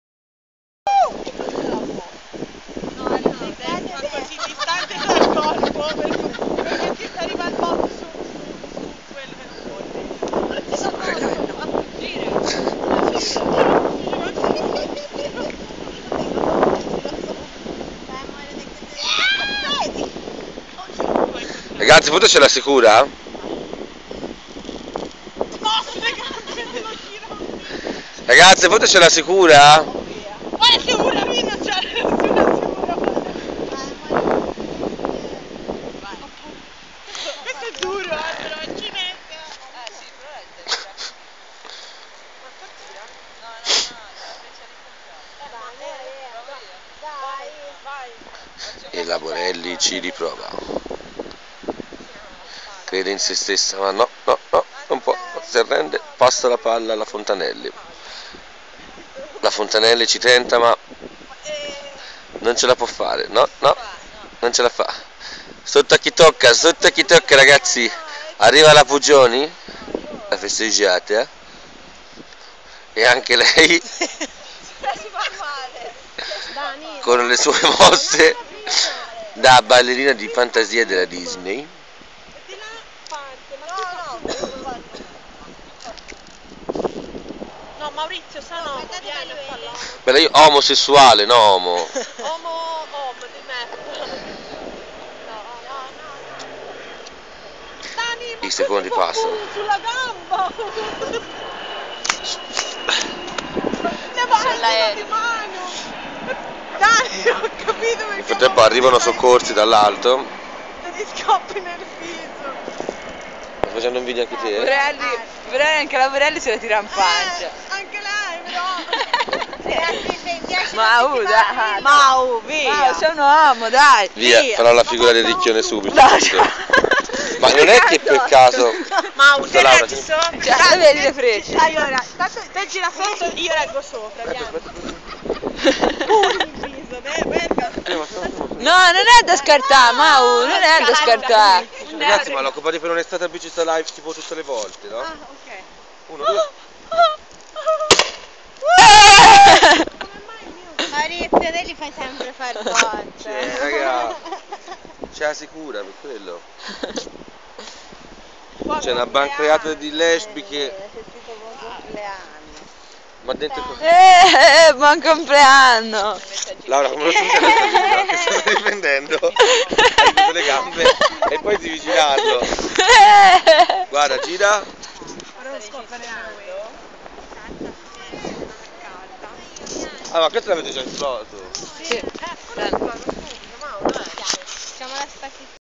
Mauriziano insegna a delle ragazze come si usa uno SPUTA CORIANDOLI
Lezioni di sputa coriandoli.